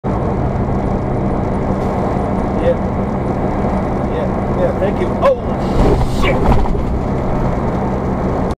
Oh 💩 Moment
Ran over some wood on the road. Fortunately nothing was damaged.